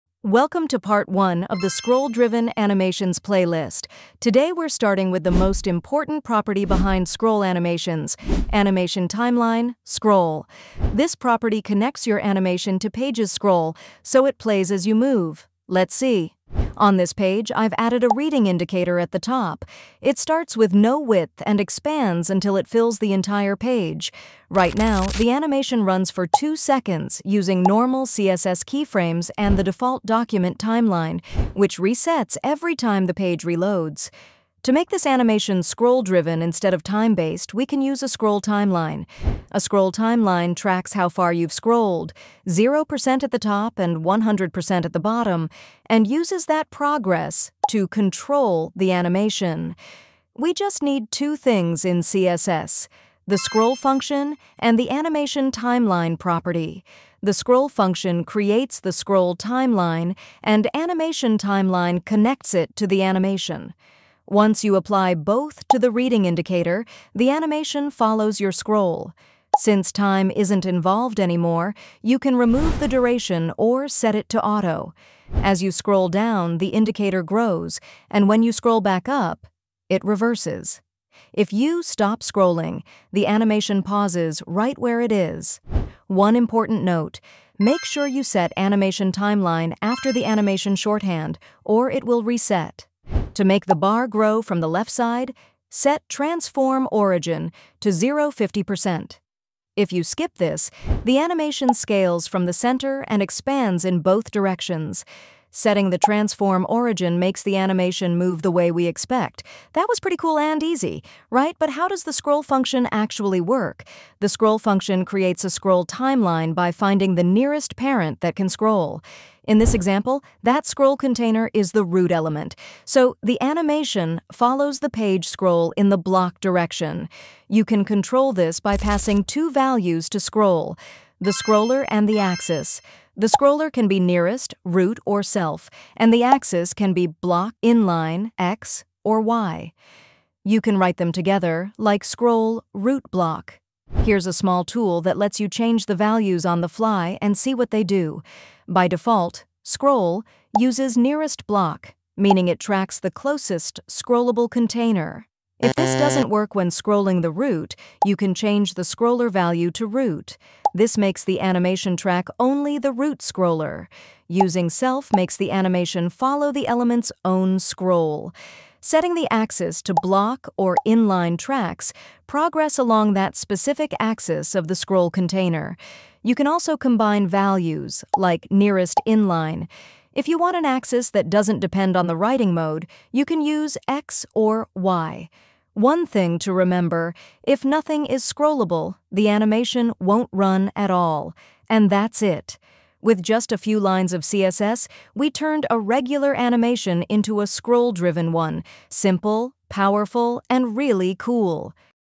Mastering CSS Scroll-Driven Animations #2 - Scroll progress timeline
Learn CSS Scroll-Driven Animations - Scroll progress timeline [Part -1]  🚀

In this video, you’ll learn how to create a scroll-based reading progress bar using pure CSS with animation-timeline: scroll().

We’ll start with a normal keyframe animation, then convert it into a scroll-driven animation using the scroll() function. You’ll see how scroll progress controls the animation from 0% at the top to 100% at the bottom, and how it moves forward, reverses, and pauses based on your scroll.

✅ What You’ll Learn:
• How animation-timeline: scroll() works
• How to use the scroll() function in CSS
• Reading progress bar animation with scroll

Watch the full playlist to master CSS Scroll-Driven Animations step by step!